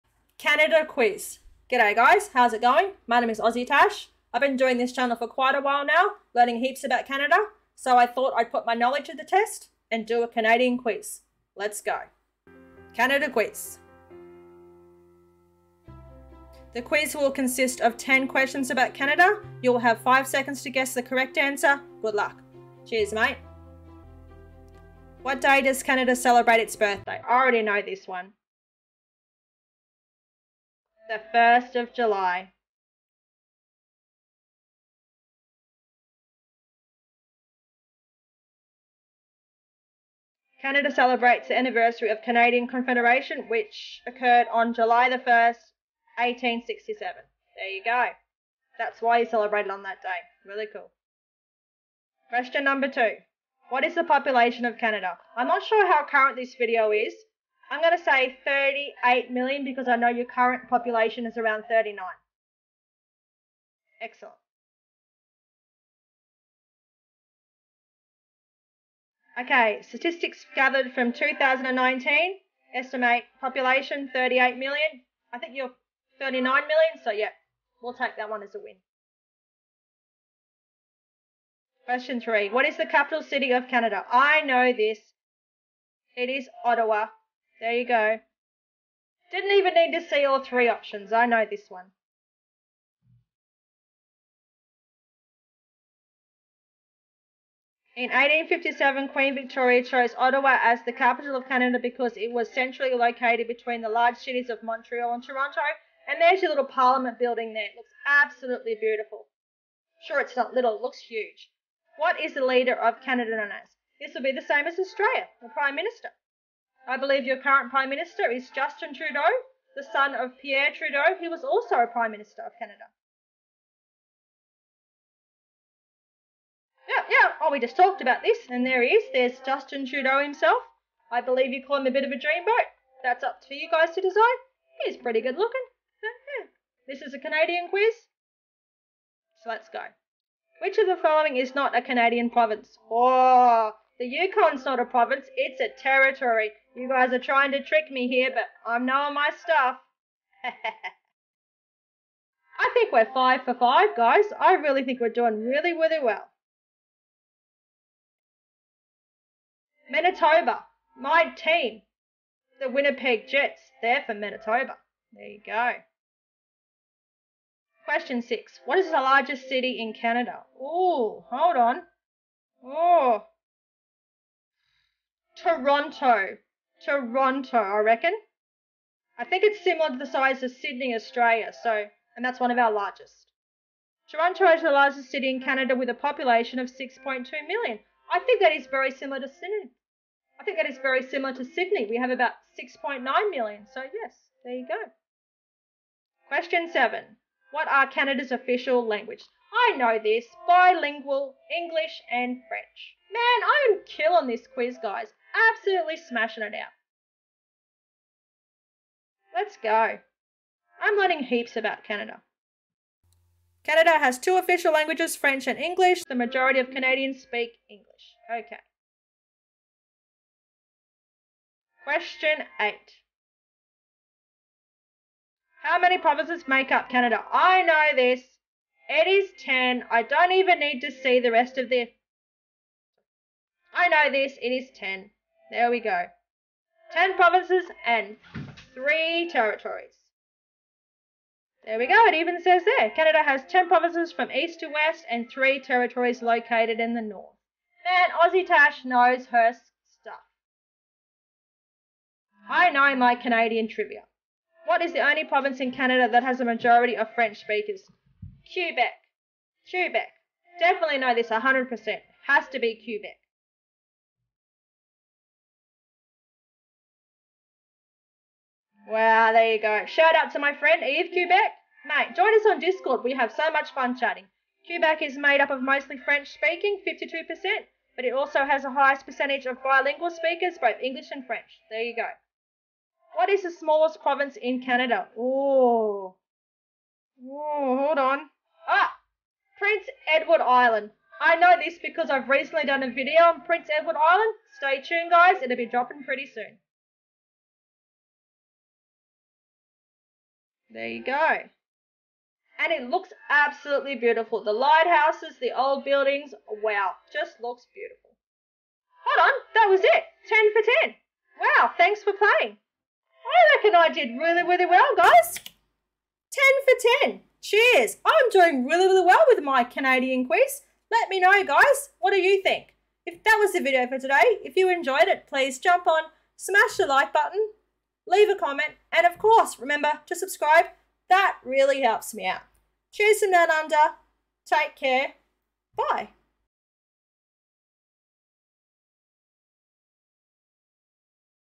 AUSSIE takes a CANADA QUIZ | Australian Reacts | AussieTash
G'day mates!!! We're back with another reaction video called 'AUSSIE takes a CANADA QUIZ' and it was a "testing' video to record. Cheers!

If you would like to send me anything in the mail, please send to:

AussieTash
Victoria Point West, QLD, 4165
Australia